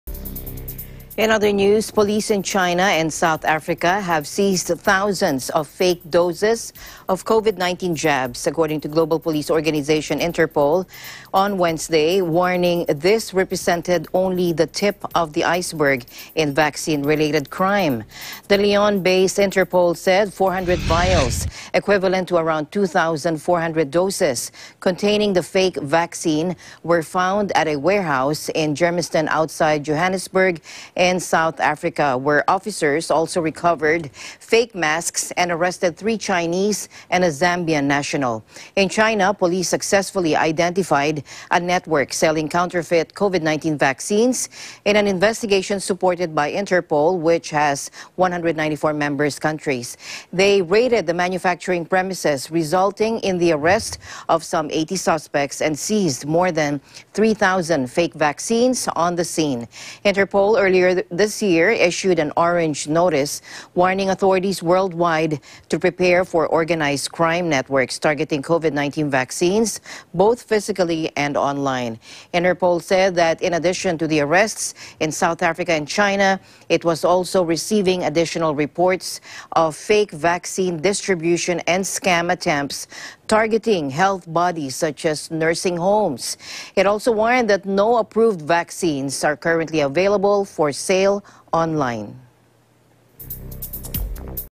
Interpol: Fake covid-19 vaccines seized in South Africa, China
Police in China and South Africa have seized thousands of fake doses of Covid-19 jabs, global police organisation Interpol said, warning this represented only the "tip of the iceberg" in vaccine-related crime.